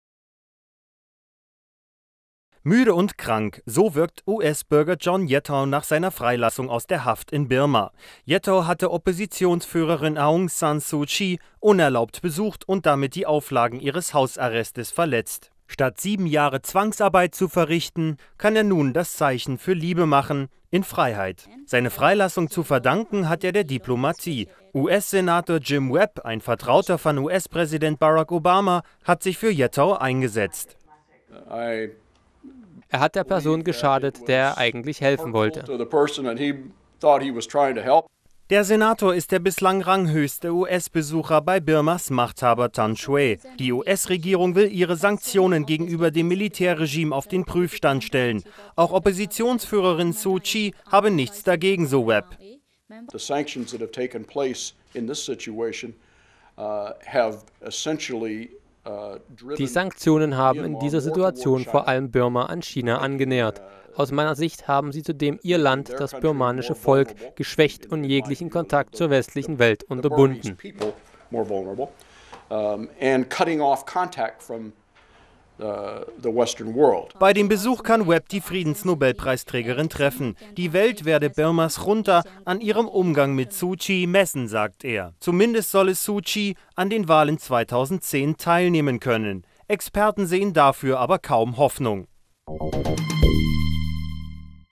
Hoffnung auf milderen Kurs gegenüber Suu Kyi in Birma
In Birma könnte die Freilassung des eigentlich zu mehrjähriger Haft verurteilten US-Bürgers John Yettaw eine mildere Politik gegenüber Oppositionsführerin Aung San Suu Kyi bringen. Experten erwarten jedoch, dass die 64-Jährige bis zu den Wahlen 2010 unter Hausarrest bleiben muss. US-Senator Jim Webb hatte in Birma die Freilassung von Yettaw erreicht und auch mit Suu Kyi sprechen können. Diese hat nach den Worten von Webb nichts dagegen, dass einige Sanktionen gegen ihr Land aufgehoben werden.